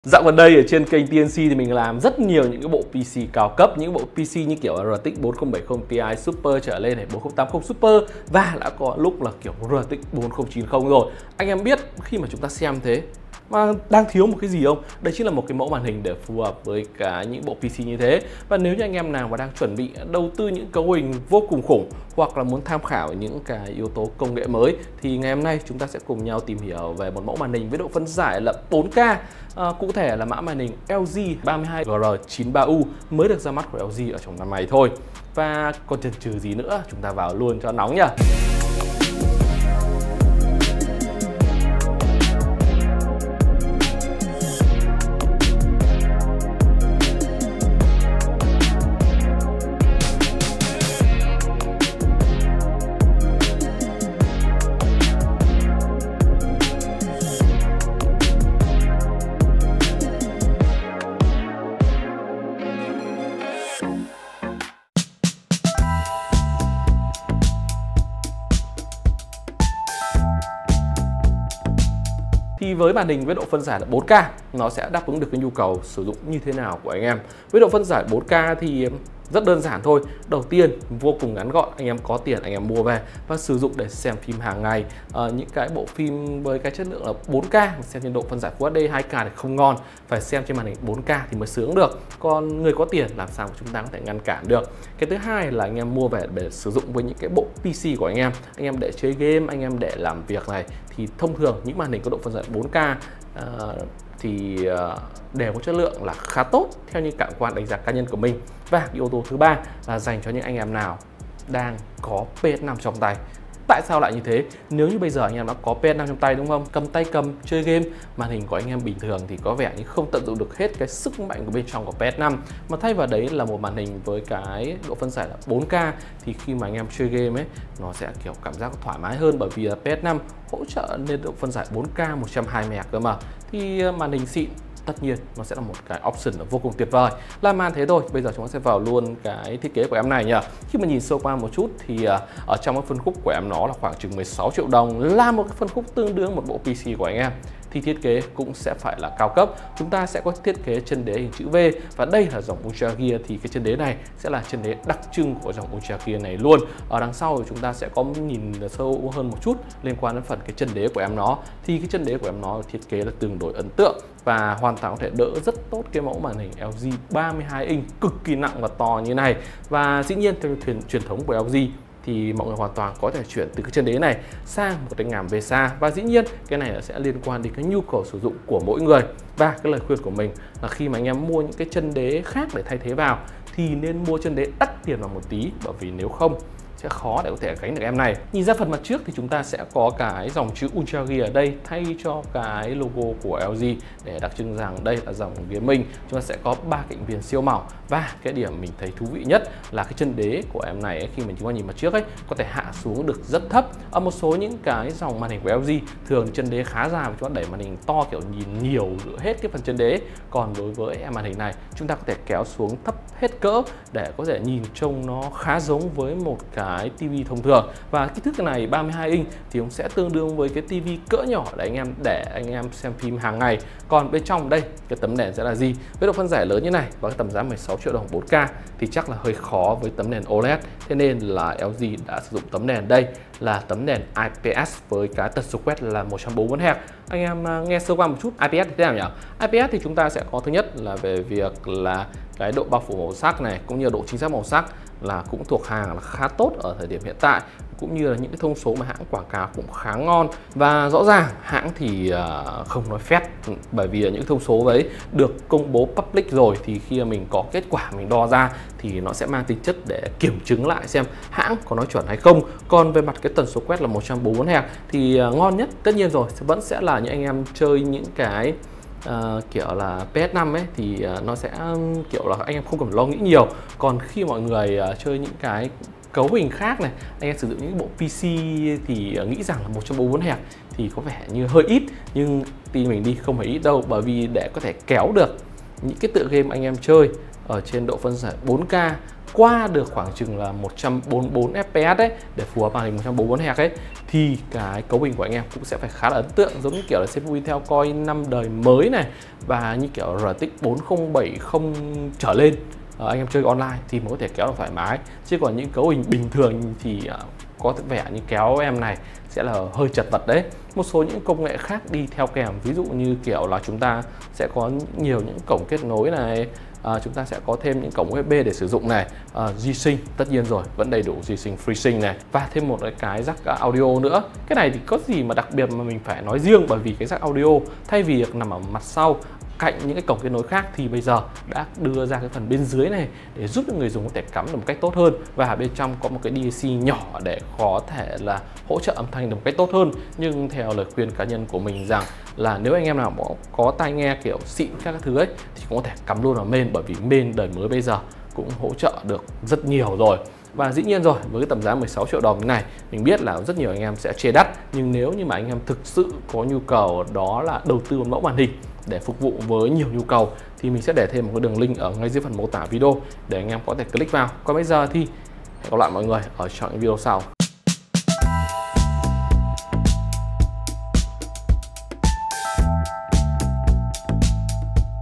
LG UltraGear 32GR93U - PS5 Slim Chơi Trên Màn 4K "SƯỚNG" Thế Nào???
Màn Hình Gaming LG UltraGear 32GR93U-B 31.5 inch / UHD / IPS / 144Hz / 1ms
Kiểu dáng màn hình: Phẳng
Tỉ lệ khung hình: 16:9
Kích thước mặc định: 31.5 inch
Công nghệ tấm nền: IPS
Phân giải điểm ảnh: UHD - 3840X2160
Độ sáng hiển thị: 320 Nits cd/m2
Tần số quét màn: 144Hz
Thời gian đáp ứng: 1ms (GTG)
Chỉ số màu sắc: 1,07 tỷ màu
Hỗ trợ tiêu chuẩn: VESA DisplayHDRTM 400 - DCI-P3 95% (Thông thường)- NVIDIA® G-SYNC® - AMD FreeSync™ Premium, vesa 100x100mm

Cổng cắm kết nối: HDMI, DisplayPort, Cổng USB chiều lên, Cổng USB chiều xuống, Đầu ra tai nghe

Phụ kiện trong hộp: Dây nguồn, Dây HDMI, Dây DisplayPort, Dây USB A sang B